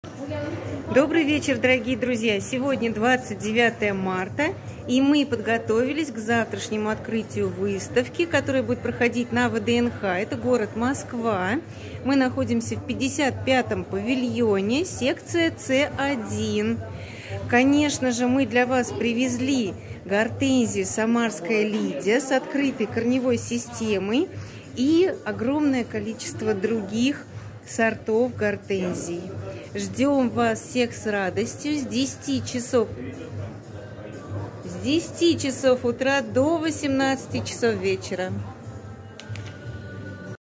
ЖДЁМ ВСЕХ НА ВЫСТАВКЕ В Г.МОСКВА
Москва, встречай! Мы привезли нашу красоту!

УЖЕ ЗАВТРА!
В г.Москва на ВДНХ с 30 марта по 3 апреля включительно состоится выставка в 55 павильоне ЭЛЕКТРИФИКАЦИЯ
 «Дача Сад Ландшафт и Малая Механизация»
наш стенд №10

Время работы выставки:

30 марта с 12:00 до 18:00
31–2 апреля с 10:00 до 18:00
3 апреля с 10:00 до 16:00

Будем рады всех видеть!
ДО ВСТРЕЧИ!

Друзья, обращаем ваше внимание! Все кто сделали заказ,  мы не привезём ваши посылки на выставку, ваши заказы приедут к вам транспортной компанией.